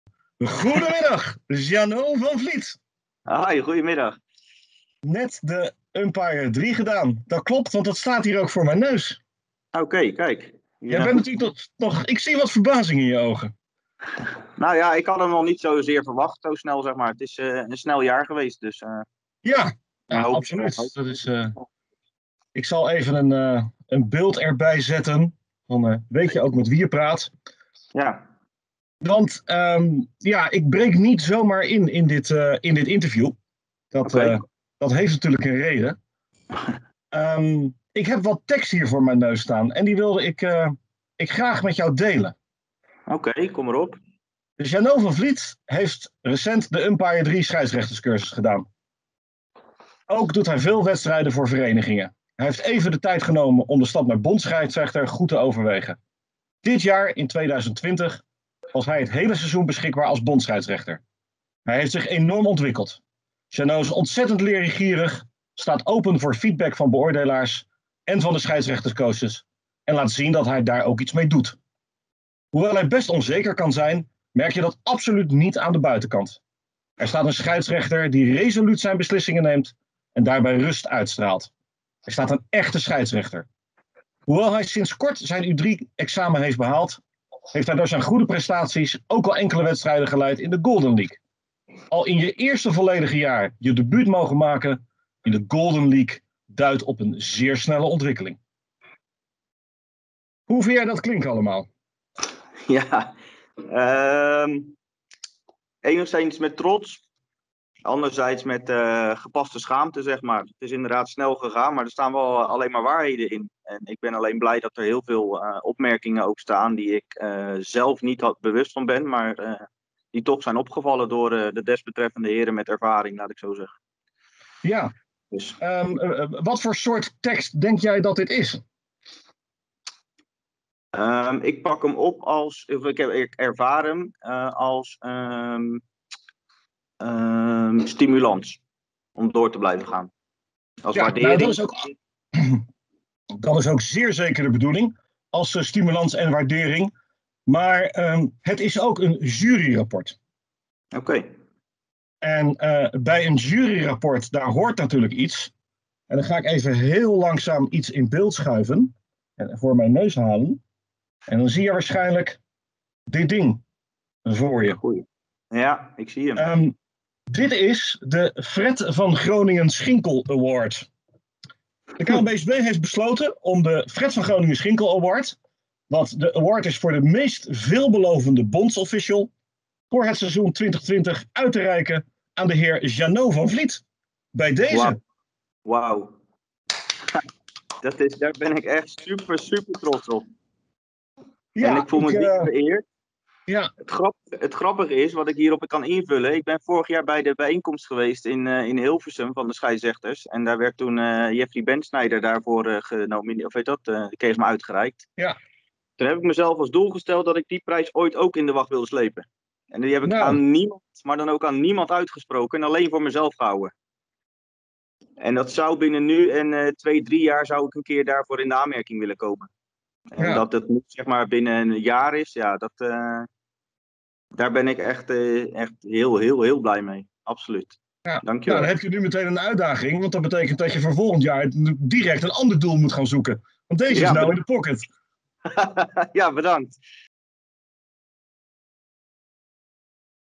KNBSB Awards 2020: Fred van Groningen Schinkel Award naar Jeanot van Vliet
De KNBSB Fred van Groningen Schinkel Award 2020 is voor Jeanot van Vliet! In deze 'virtuele awarduitreiking' wordt Jeanot verrast door Norbert van Oosterhout, die inbreekt in een zogenaamd interview met de KNBSB om hem het goede nieuws te vertellen. De Fred van Groningen Schinkel Award gaat altijd naar de meest veelbelovende KNBSB bondsofficial.